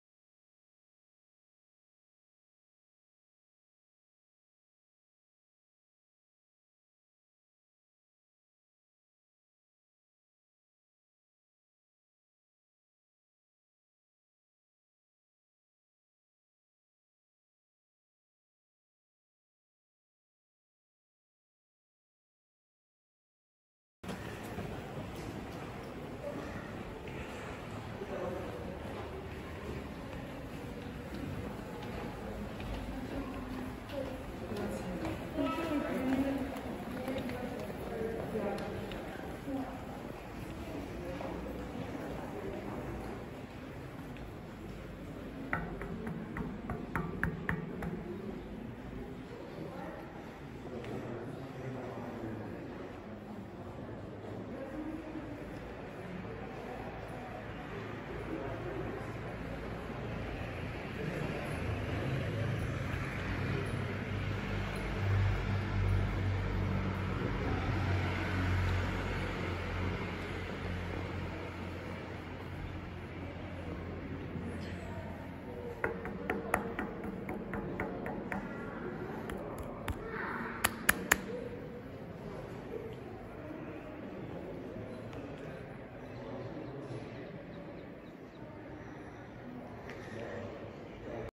Le antiche campane dei Musei Vaticani - "Rintocchi"
Stato della Città del Vaticano (V)
Musei Vaticani

2 Campane antiche all'interno dei Musei, che prima appartenevano alla Basilica di San Pietro
Fonditori Pisani XIII secolo

Elenco delle suonate:
0:24 Panoramica e "Suono" della Campana 1
0:54 Panoramica e "Suono" della Campana 2

Le Campane non si sentono molto, questo video è stato fatto principalmente per "illustrarle".